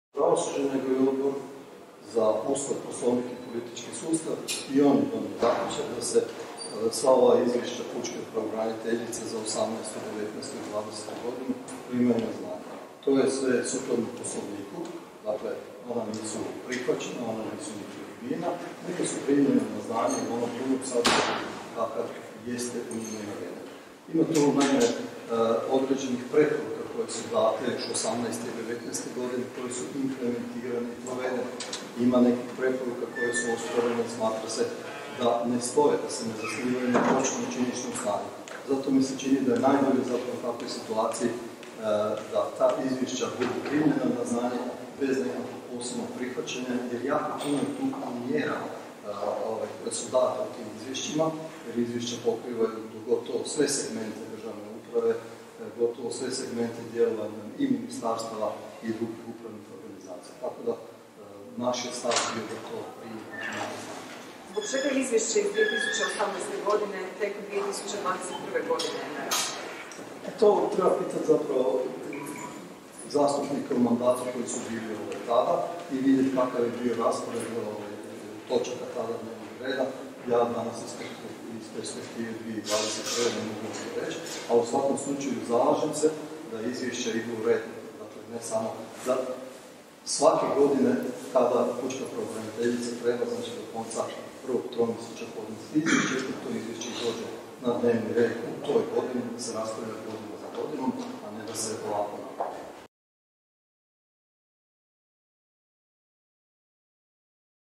Dražen Bošnjaković komentirao raspravu o izvješću pučke pravobraniteljice
Saborski zastupnik HDZ-a Dražen Bošnjaković komentirao je u Saboru raspravu o izješćima pućke pravobraniteljice.